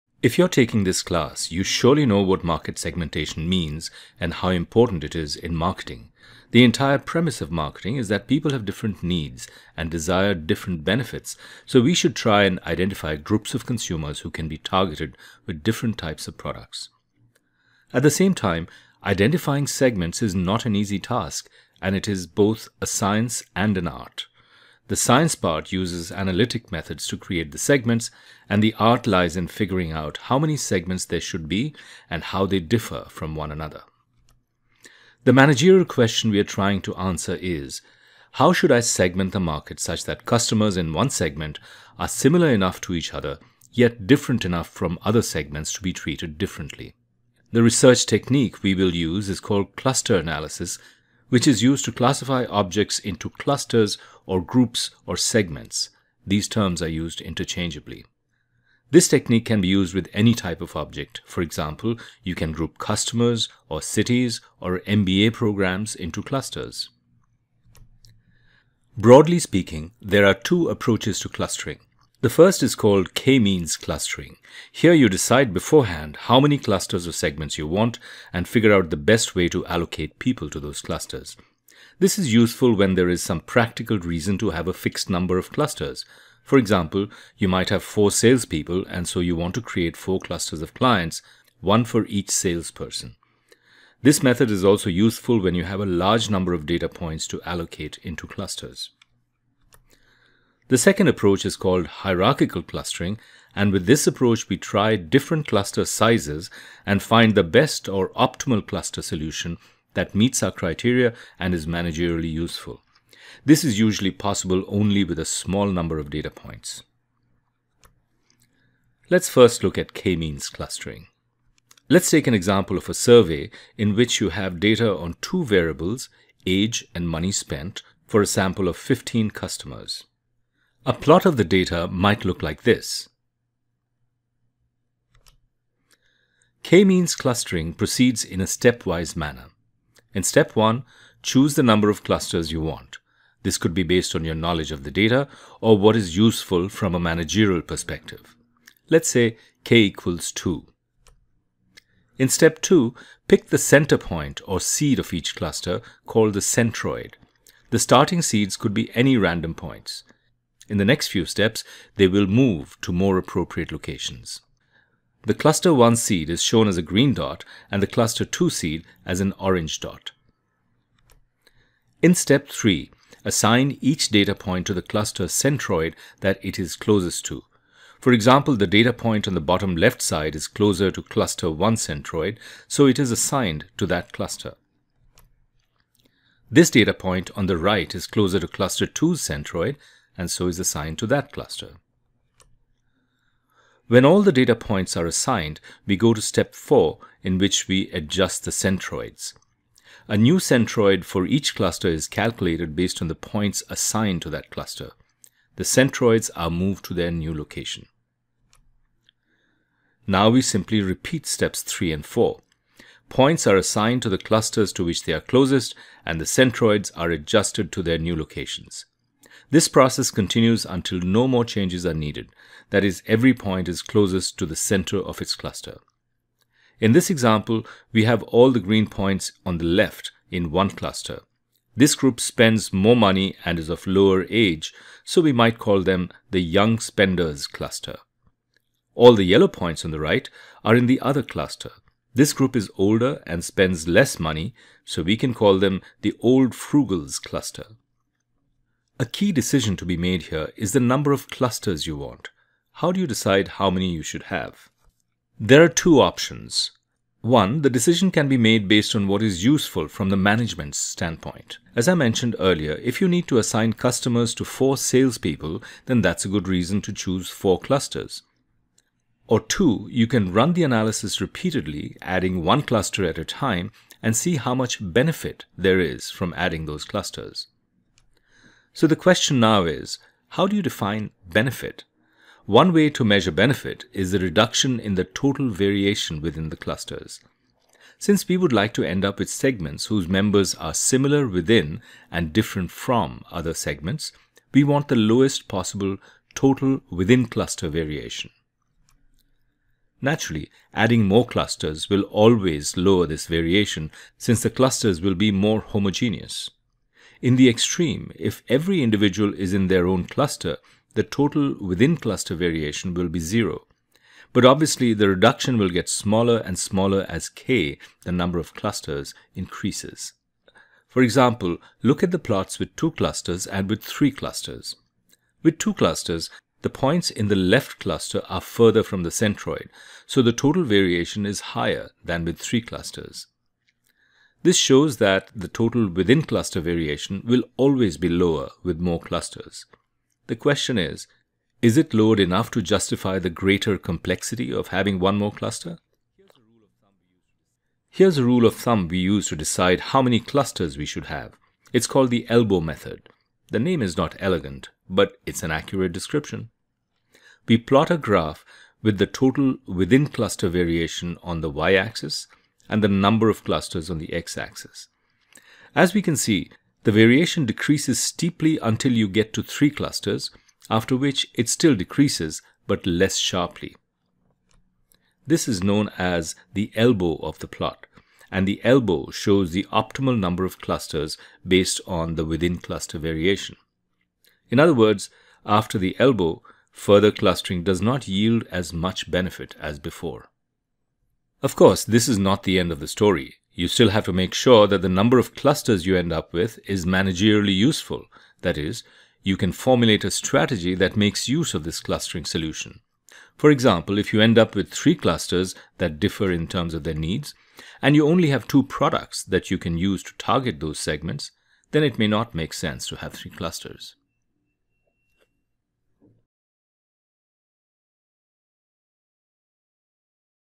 Market Segmentation using Cluster analysis
Part of a series of lectures for a marketing research class at Baruch College @thedigitalprof